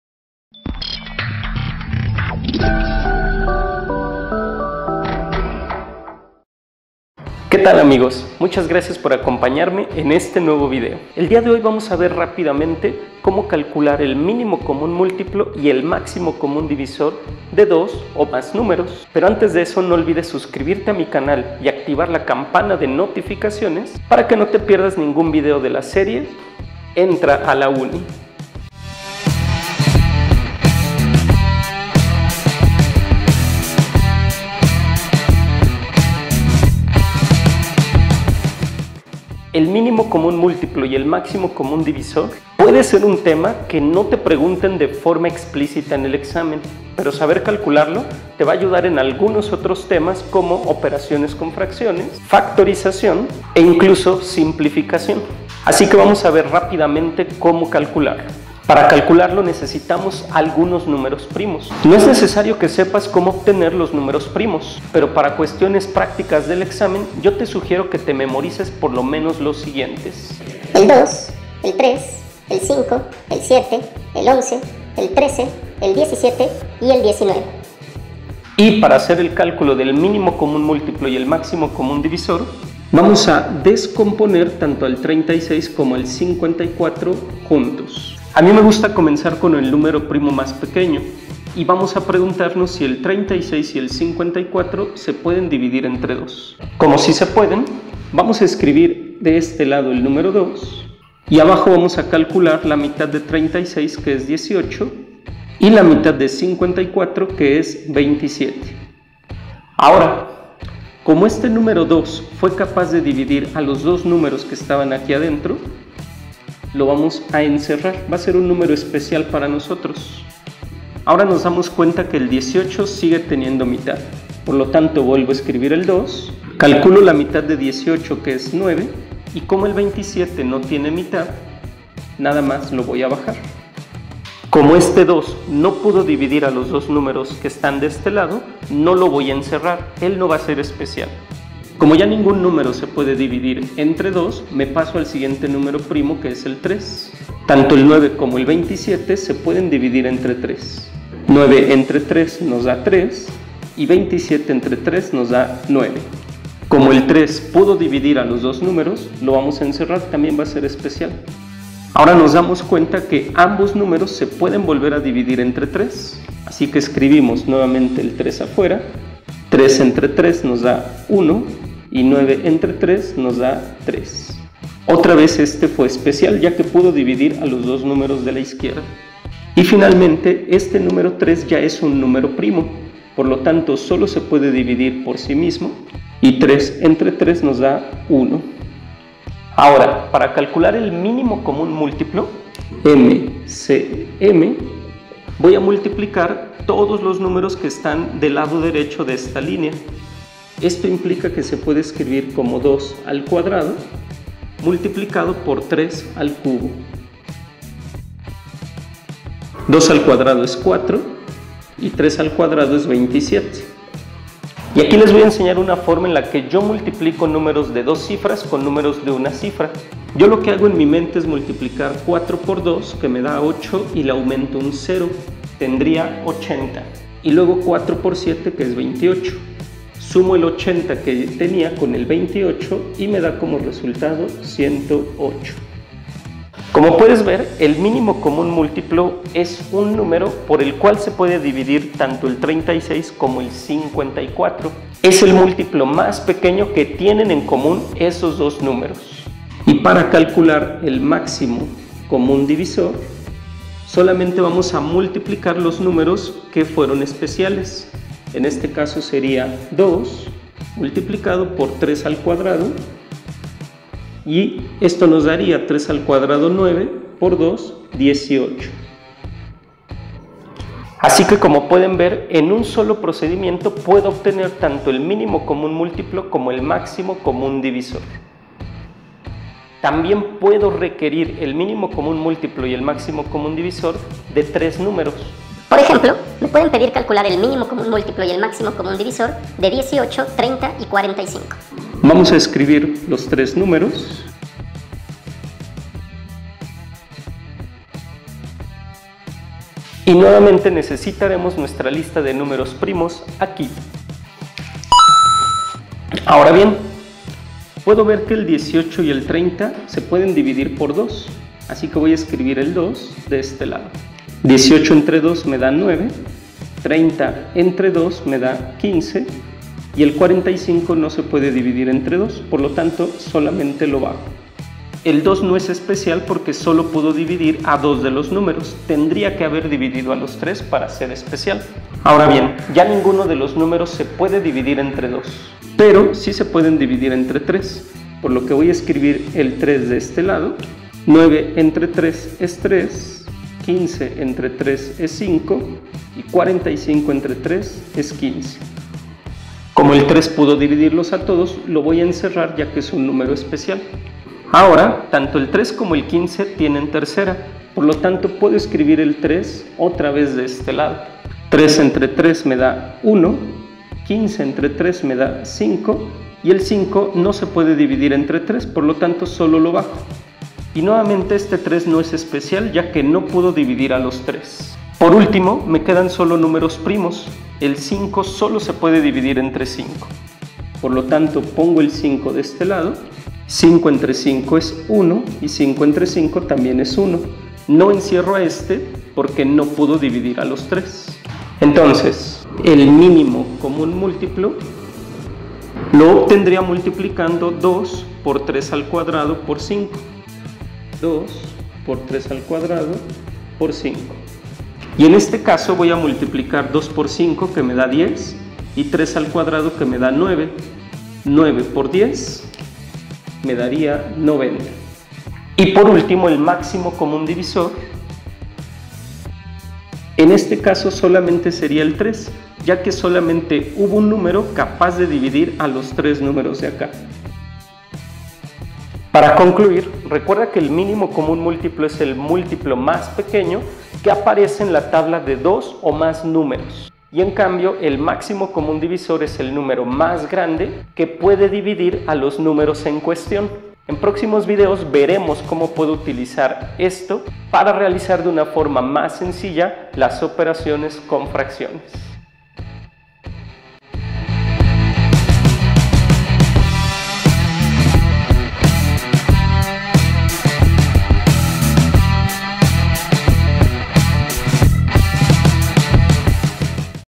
EntraALaUni 2 ||💥Como calcular el MÍNIMO COMÚN MÚLTIPLO y el MÁXIMO COMÚN DIVISOR 💥[ mcm y MCD ] 🌟
En éste vídeo aprenderemos a calcular de forma sencilla el mínimo común múltiplo y el Máximo Común Divisor de 2 o 3 números.